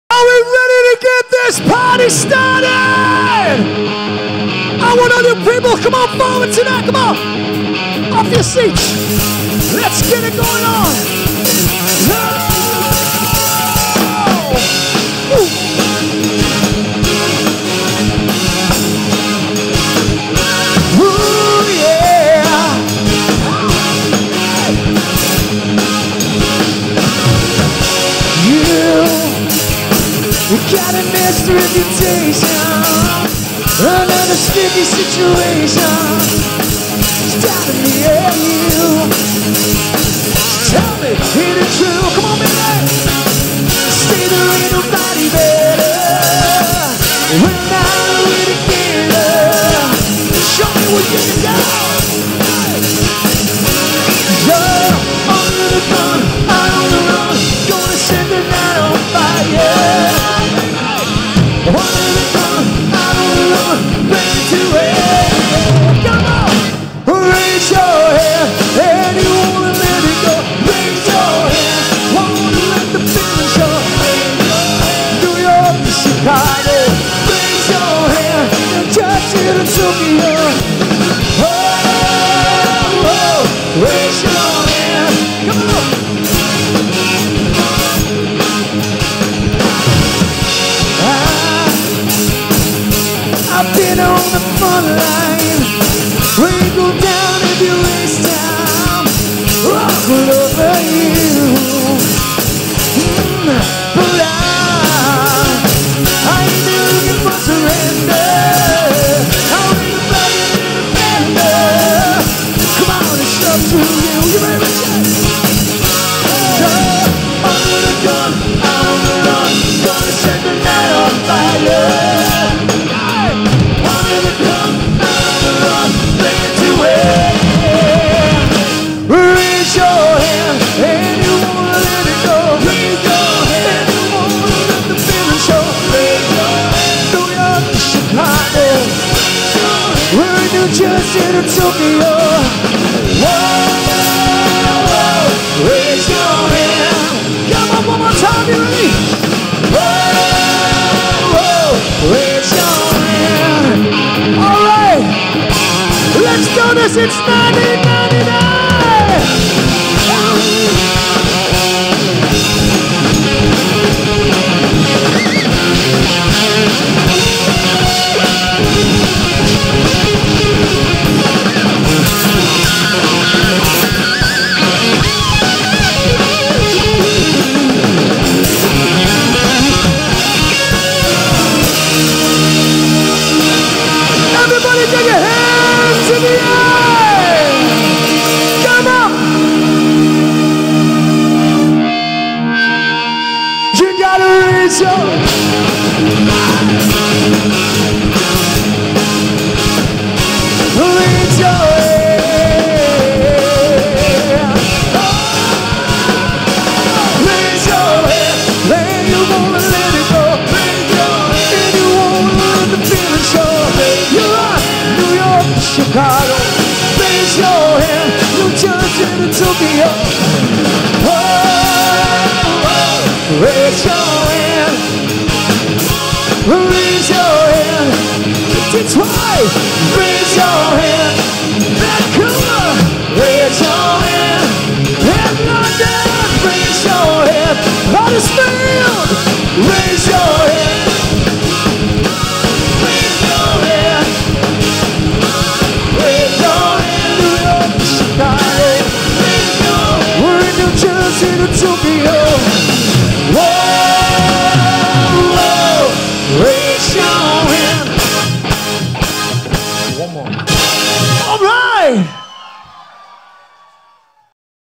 BON JOVI FOREVER AT WANNASEE HUDDERSFIELD 2016
The fantastic tribute band Bon Jovi Forever at Wannasee Festival Huddersfield 2016.